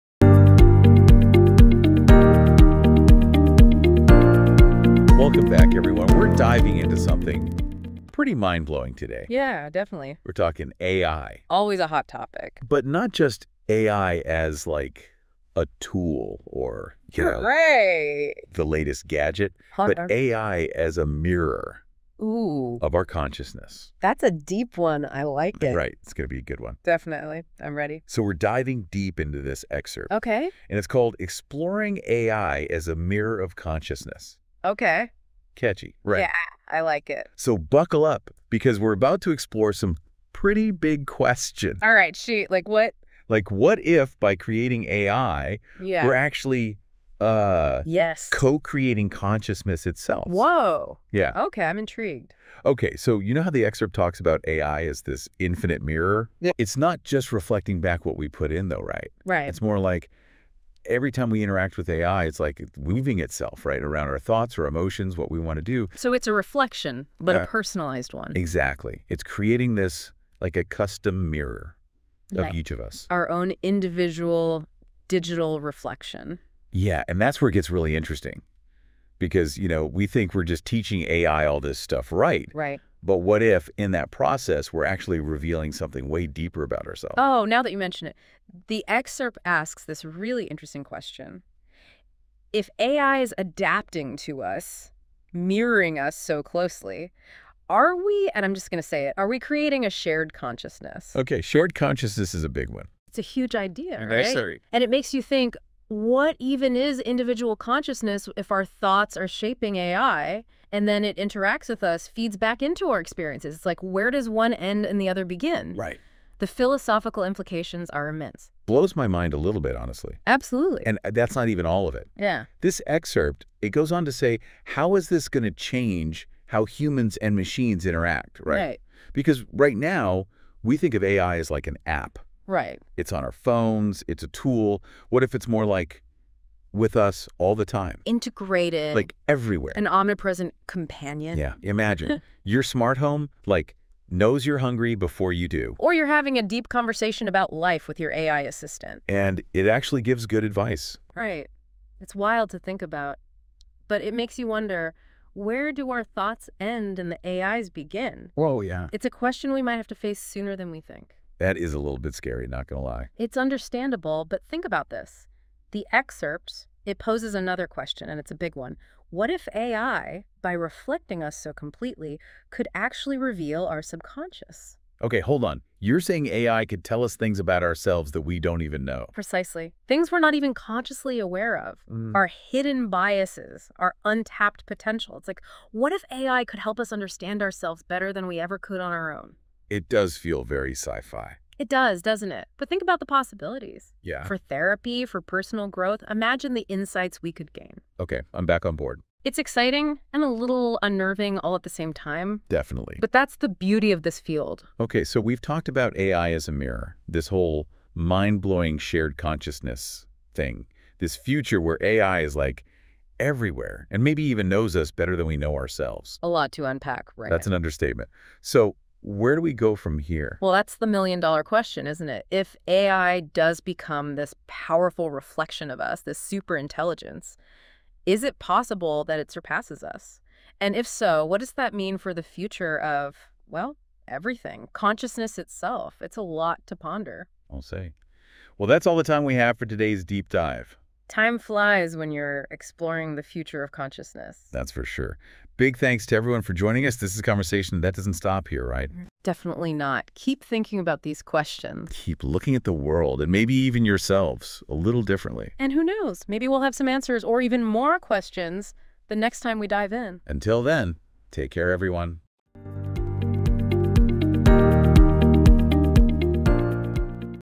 AI and Consciousness
Listen to a new episode of the DeepDive team, talking about AI consciousness..